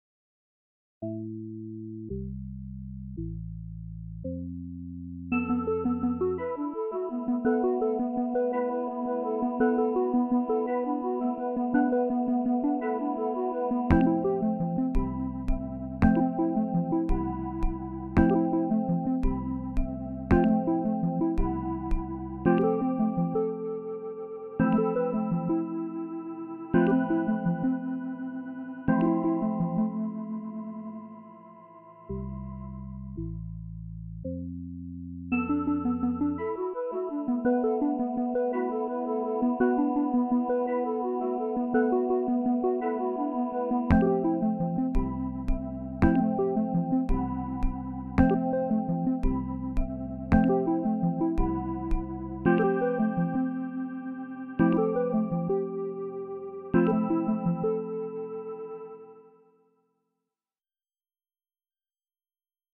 Track with Two New OP-Z Synth Engines
The two new synth engines that just came out are pretty inspiring.